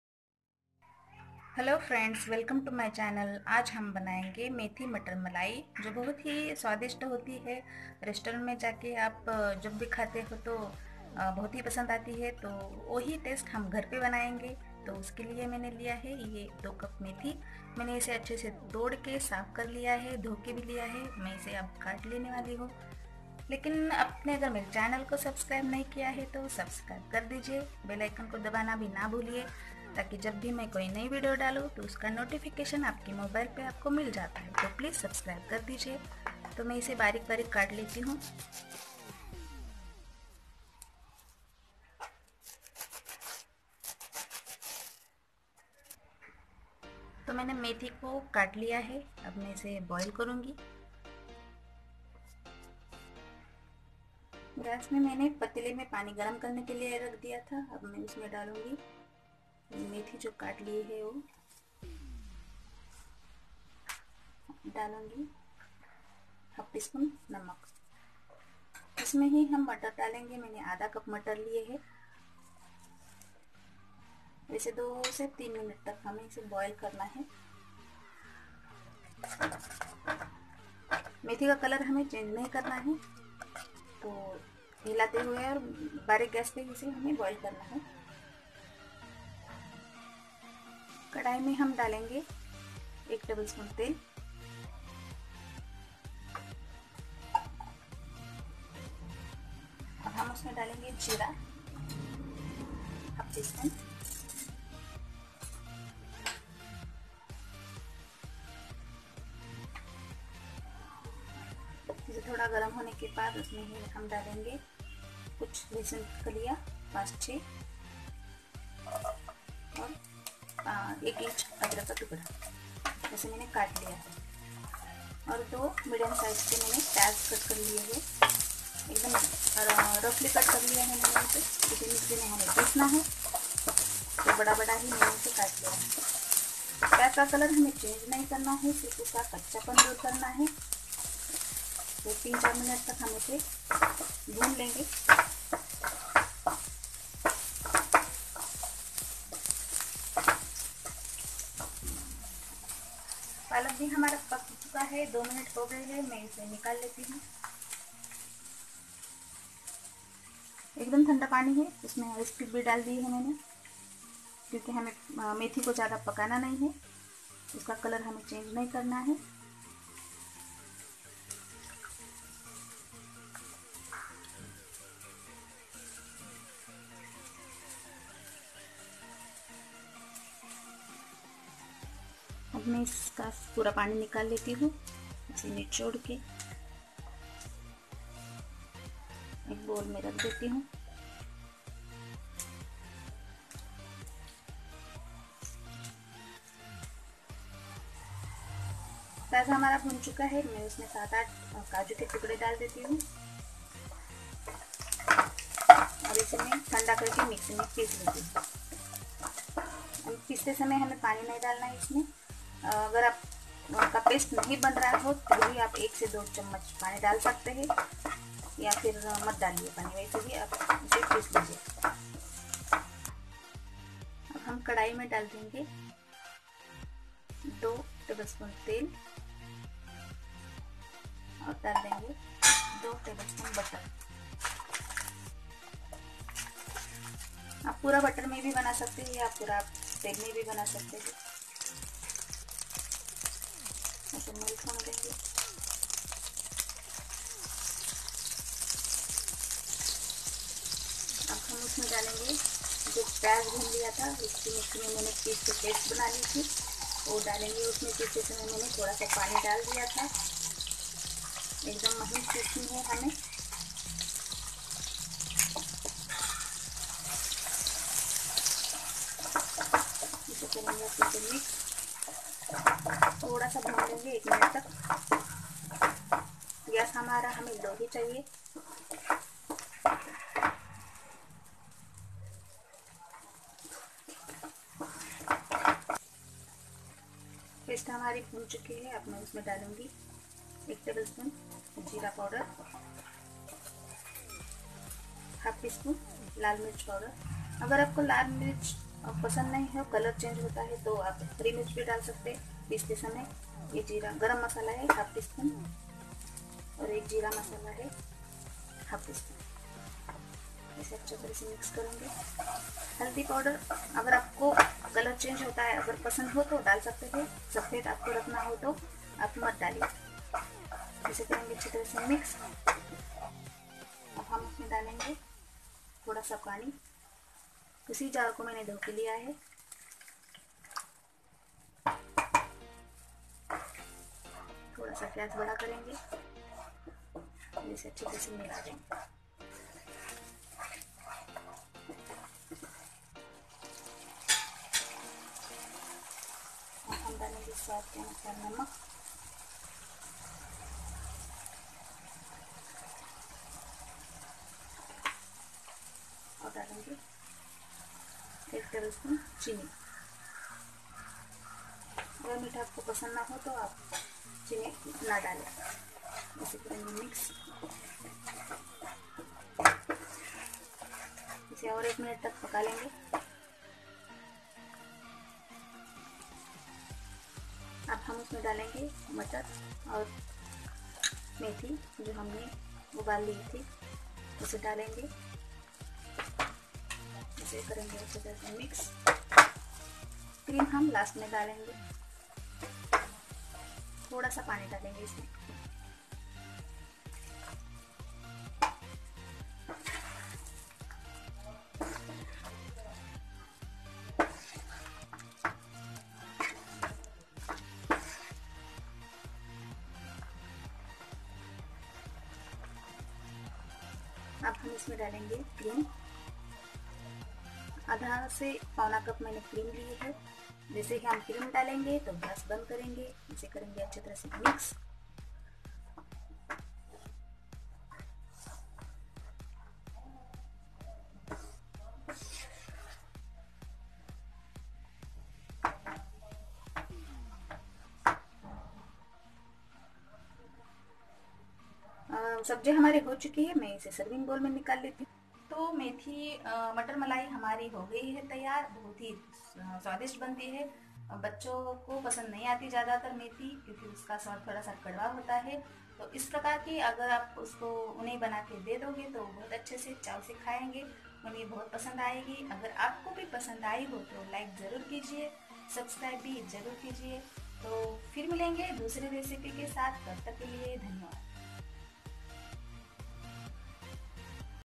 मेथी मटर मलाई अब रेस्टोरंट स्वाद जैसी | Now Make it like restaurant methi matar Malai | Food Corner
Ingredients

Methi= 2 cup
Matar= half cup
Onion= 2
Jeera= half tea spoon
Dhaniya powder= 1 tbs
Jeera powder= half tbs
Red chilli powder= half tbs
Garam masala= half tbs
Fresh cream= ⅓ cup
Oil
Salt
Water